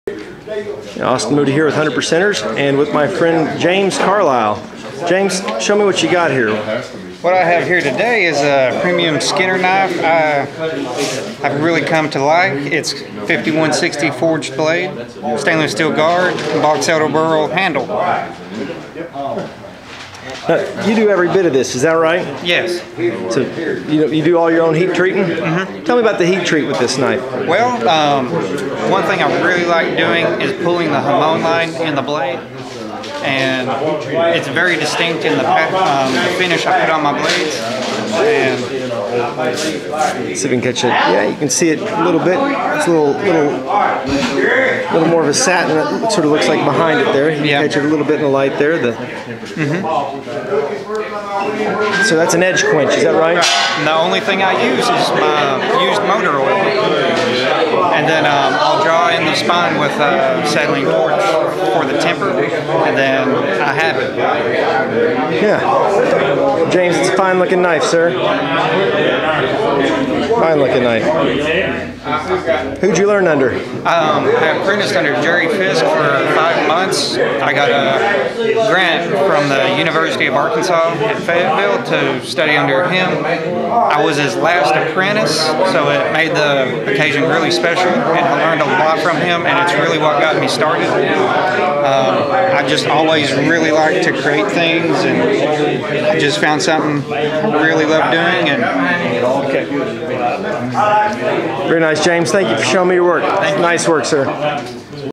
100 Percenters presents: Talented young knife maker - James Carlyle
James Carlyle is the last apprentice of fellow 100 Percenter Jerry Fisk, MS.  We met up with James at Uncle Al's monthly get together in DeQueen, Arkansas.   James makes a fine looking knife - Keep an eye on this young maker - we expect great things.